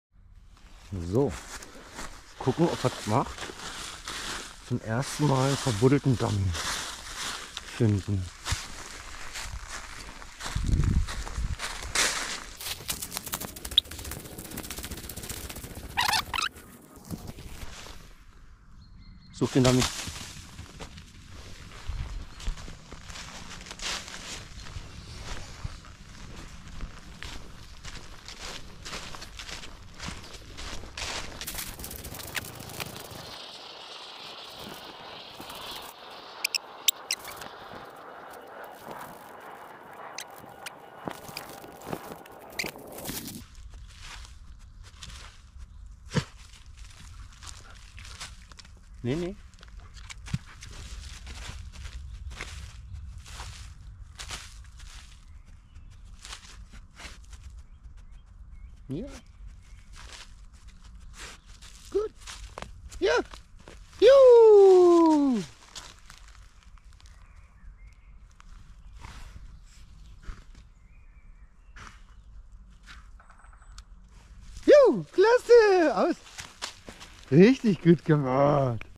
pudel dummy hundesport standardpoodle
Dummy Arbeit mit dem Pudel .
Heut durfte rumo zum 1. mal einen vergrabenen Dummy suchen. positiv zu bewerten ist die Platzablage,das eifrige selbständige suchen. nicht so schön war die Abgabe und die leichte Hilfestellung meinerseits.
falls ihr Tipps, Feedback geben möchtet gern in die Kommentare (evtl zu erwähnen,die sucharbeit wurde von mir aufgebaut,ohne professionellen Trainer,vergibt mir wenn die Form oder Ablauf nicht der Norm entspricht)